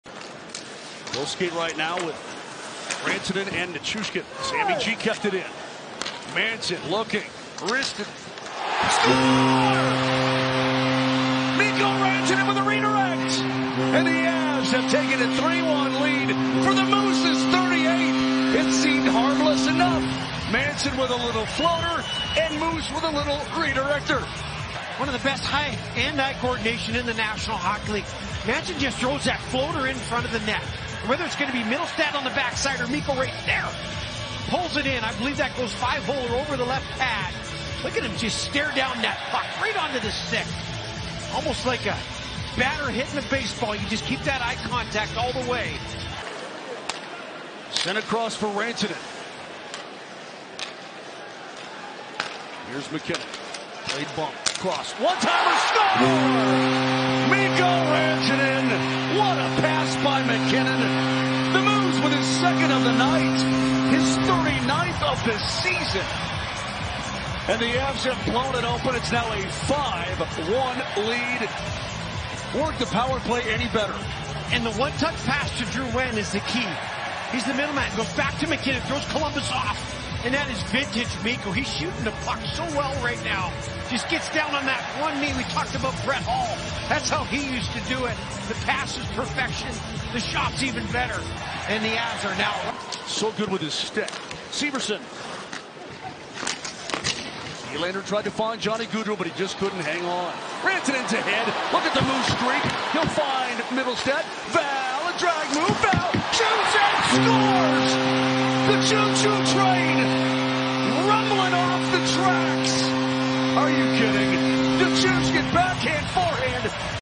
Rantanen tekee kaksi maalia ja syöttää yhden 🫎 Rantanen's scores twice in three-point game
Mikko Rantanen heiluttaa Blue Jacketsin maaliverkkoa kahdesti ja alustaa Valeri Nitshushkinin osuman Avalanchen 6-1 voitossa. | Mikko Rantanen picked up a pair of goals before adding an assist on another from Valeri Nichuskin in a 6-1 Avalanche win

NHL Europe palvelee jokaista NHL-fania Euroopassa. Klikkaa isoa punaista Tilaa-painiketta ja pysyt perillä NHL:n tuoreimmista tapahtumista.